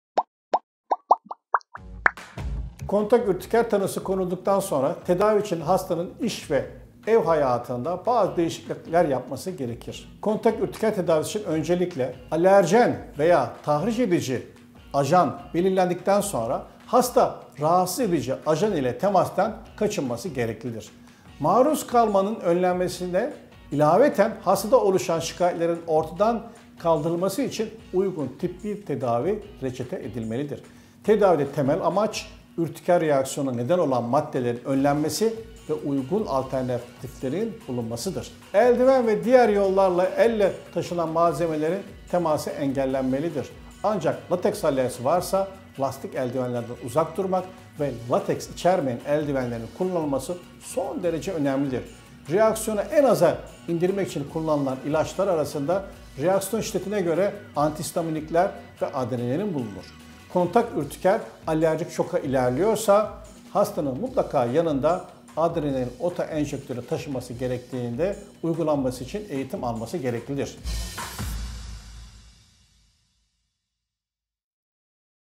Kontakt Ürtiker (Temas Ürtikeri) Tedavisi Nasıldır? - Prof. Dr. Ahmet Akçay
Kontakt ürtiker tanısı konulduktan sonra tedavisi için hastanın iş ve ev hayatında bazı değişiklikler yapması gereklidir.

Kontakt ürtiker tedavisi için öncelikle

Alerjen veya tahriş edici madde belirlendikten sonra, hasta, rahatsız edici ajan ile temastan kaçınması gereklidir.

Maruz kalmanın önlenmesine ilaveten hastada oluşan şikayetlerin ortadan kaldırılması için uygun tıbbi tedavi reçete edilmelidir.

Tedavide temel amaç, ürtikeryal reaksiyona neden olan maddelerin önlenmesi ve uygun alternatiflerin bulunmasıdır.

Eldiven veya diğer yollarla elle taşınan malzemeleri n teması engellenebilir, ancak lateks alerjisi varsa lastik eldivenlerden uzak durmak ve lateks içermeyen eldivenlerin kullanılması son derece önemlidir. Reaksiyonu en aza indirmek için kullanılabilen ilaçlar arasında reaksiyon şiddetine göre antihistaminikler ve adrenalin bulunur. Kontakt ürtiker alerjik şoka ilerliyorsa hastanın mutlaka yanında adrenalin otoenjektörü taşıması gerektiğinde uygulaması için eğitim alması gereklidir.